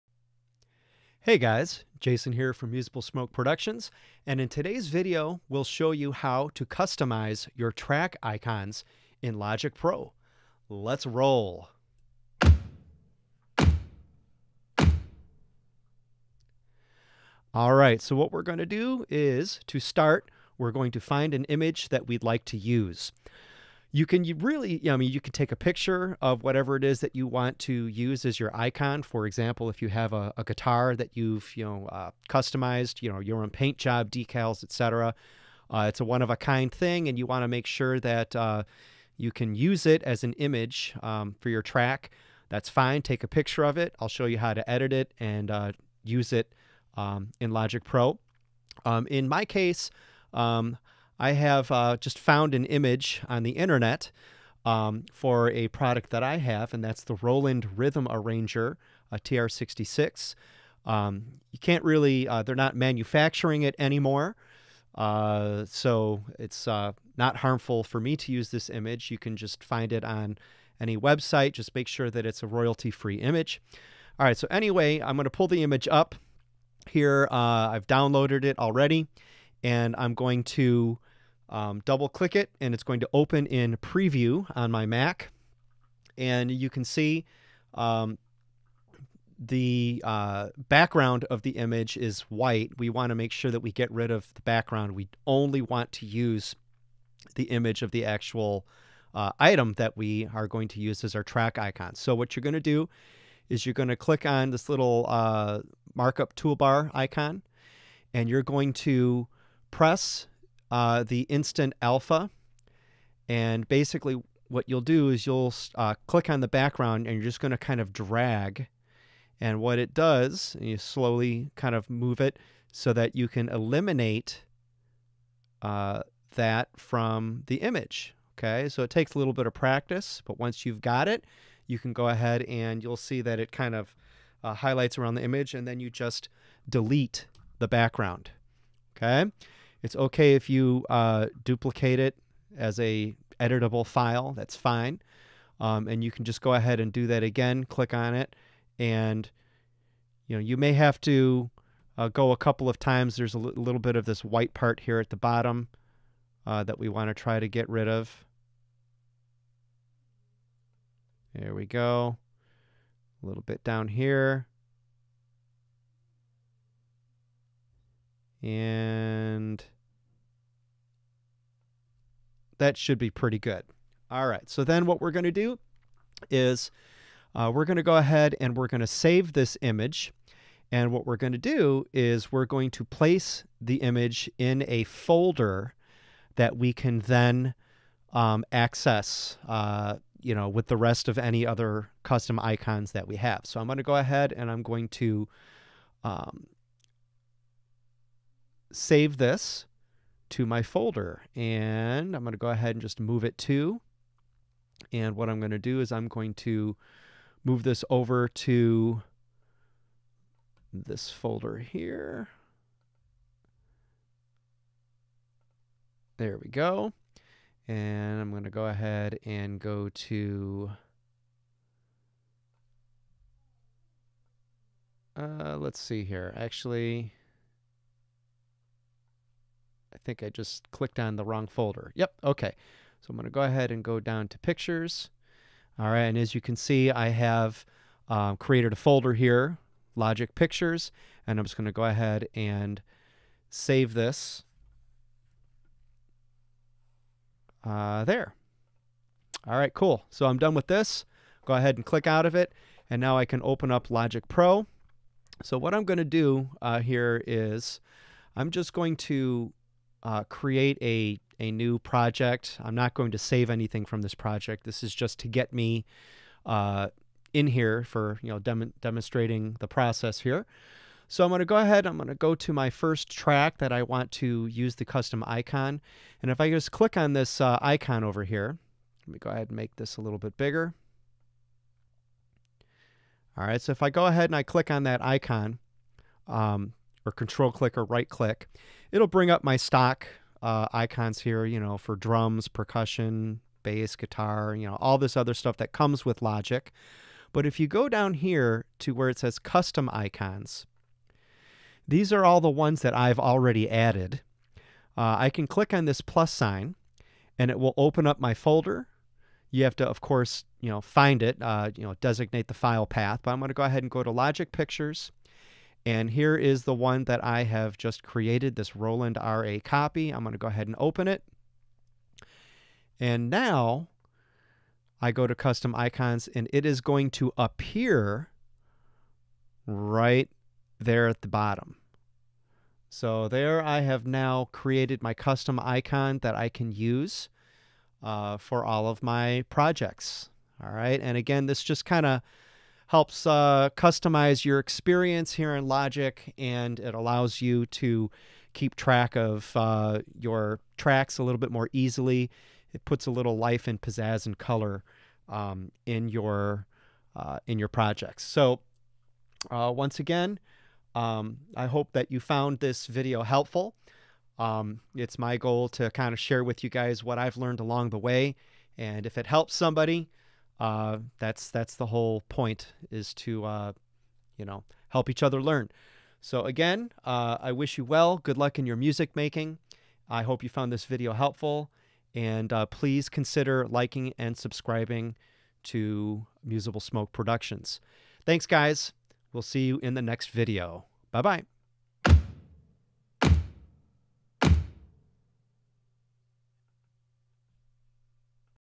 How to create custom track icons in Logic Pro
In this quick tip video, we'll learn how to create custom track icons  in Logic Pro.

I hope this video is helpful to you!
-- Jason @MSP